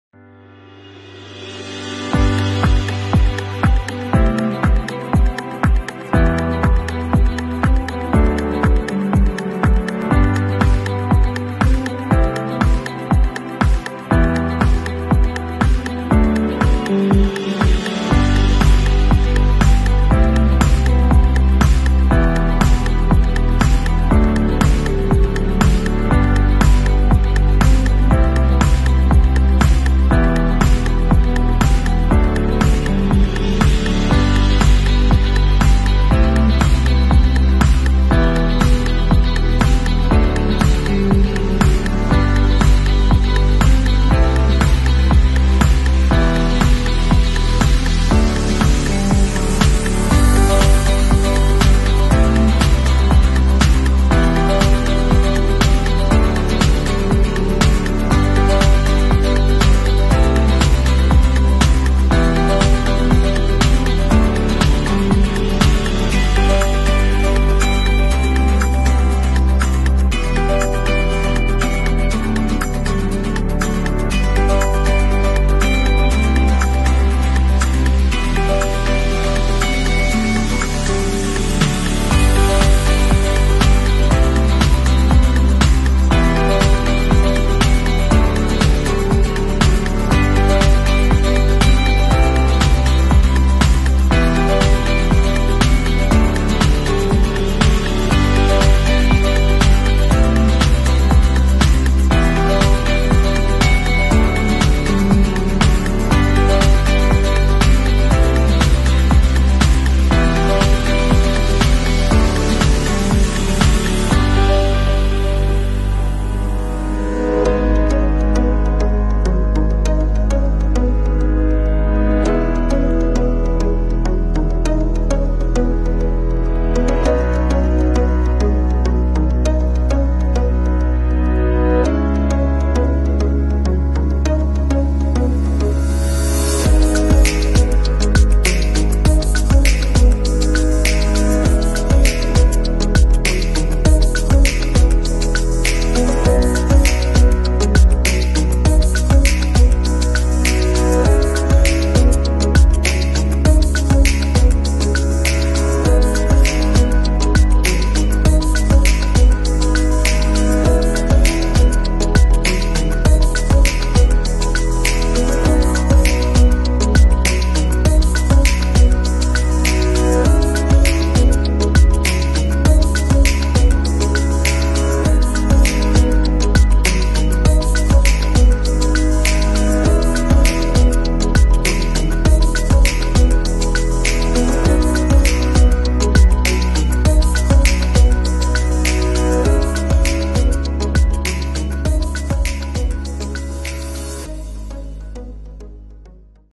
10 MOST COMMON CAMPING KNOTS
Here are 10 of the most used knots used in camping. Some are very  strong and dependable and easy to untie.